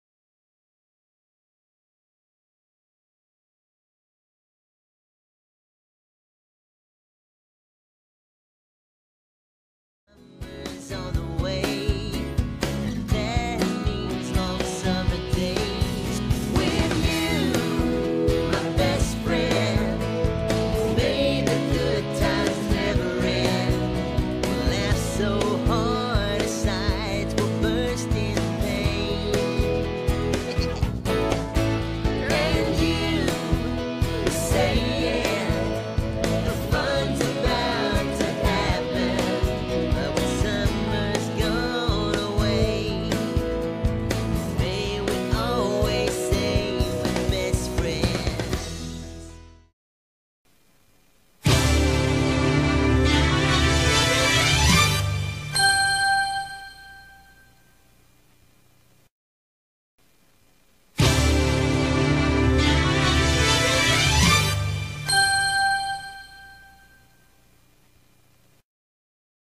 Small Showroom Tour! Chuck E Cheese Redwood City. May 2020
Bored in quarantine? Fer no more here's a small showroom!

Make sure to buy our $5 or less menu at your local location!